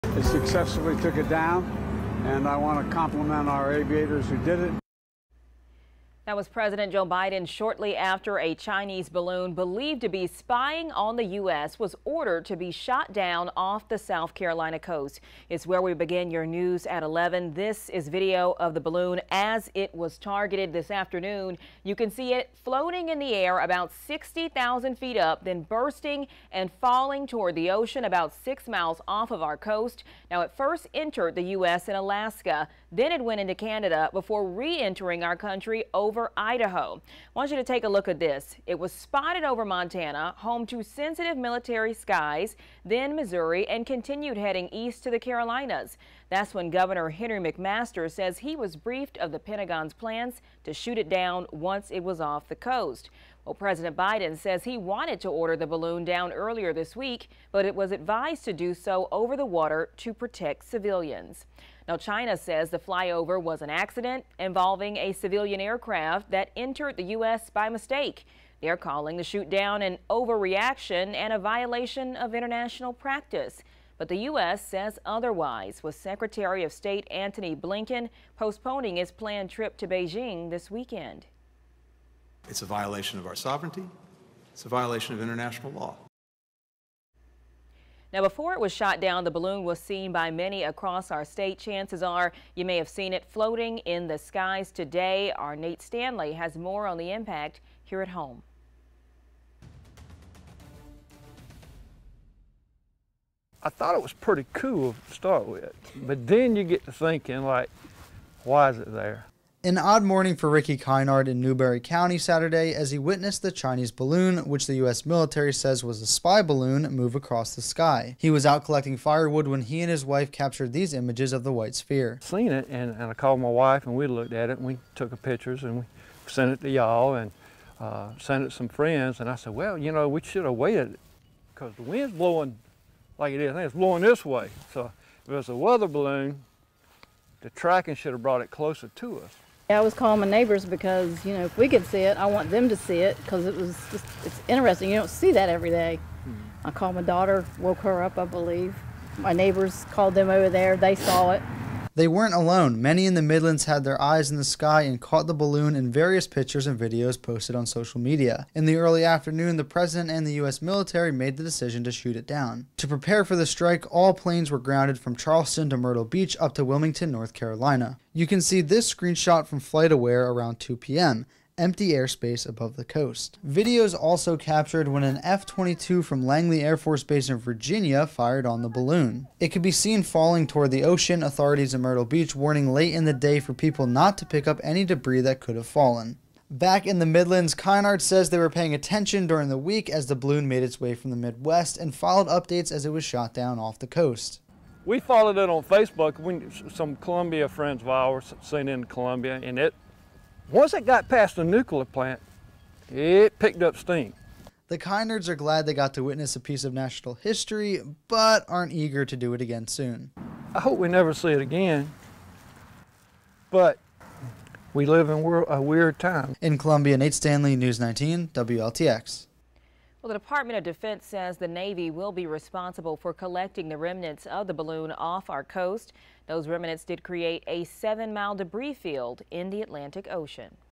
Chinese balloon shot down near Myrtle Beach, South Carolina
The U.S. military on Saturday shot down a suspected Chinese spy balloon off the Carolina coast after it traversed sensitive military sites across North America.